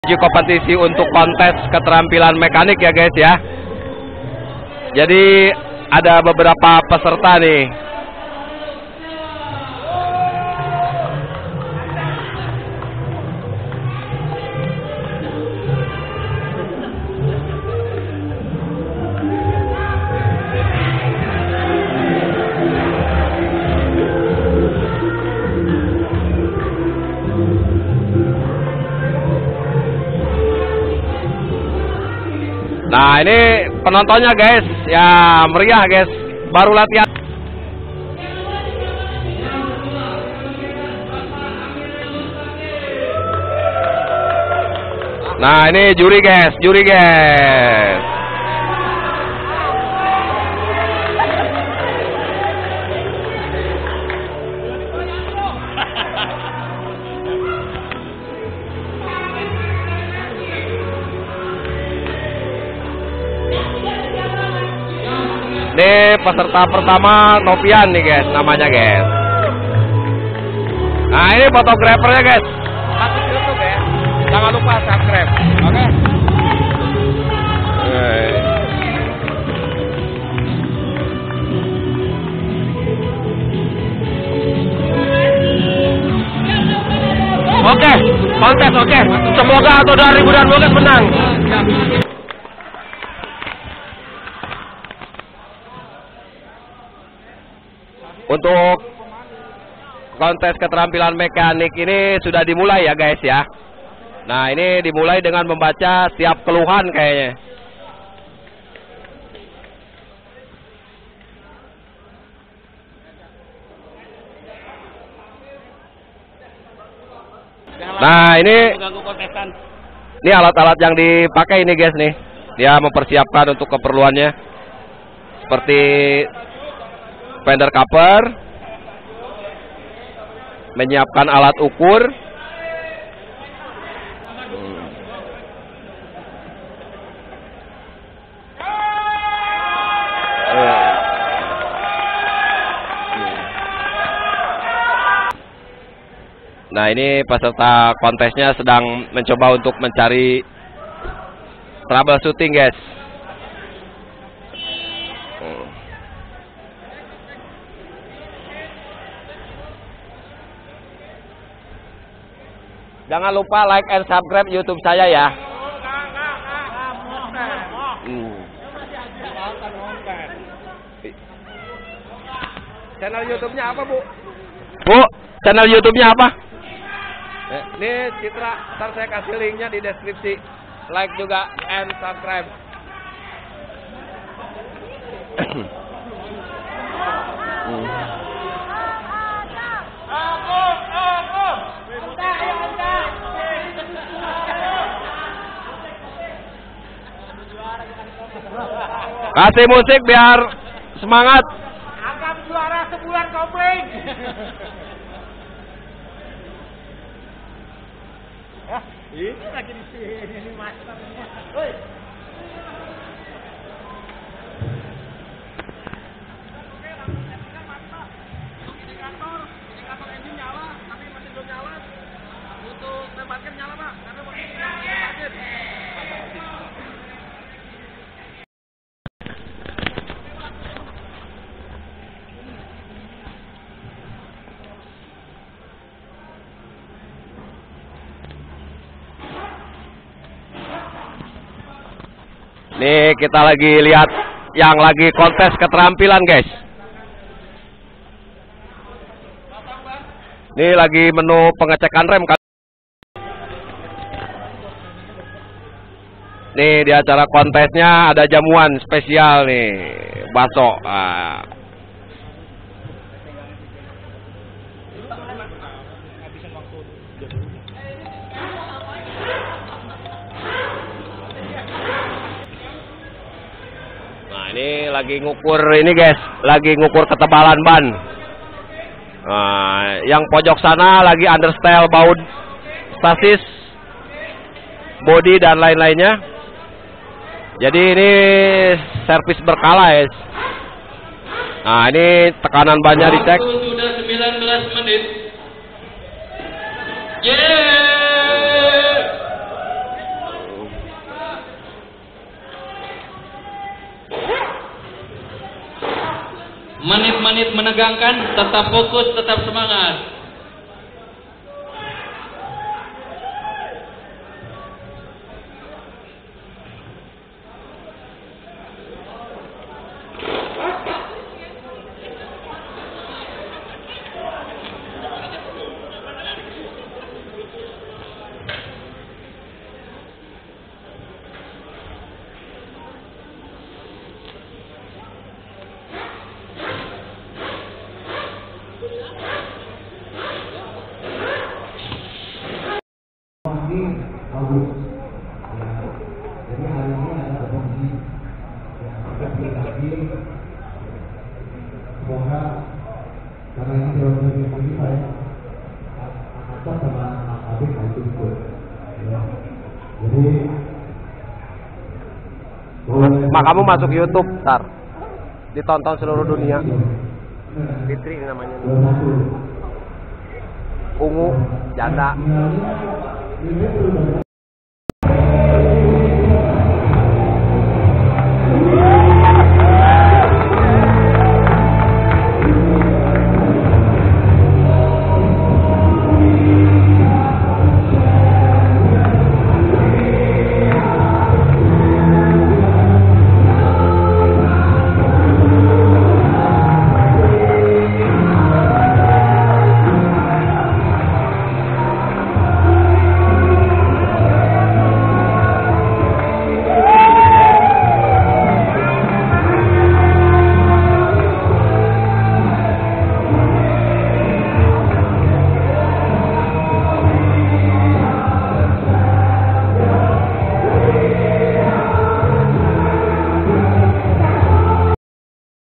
Kontes Mekanik Toyota Auto2000 Daan Mogot
DALAM RANGKA MELATIH KESIAPAN MENTAL DAN PENGUASAAN MATERI ,TROUBLE SHOOTING.
MENJELANG KONTES ANTAR CABANG SE DKI .

Mohon Doa dan Support nya ya gaessss...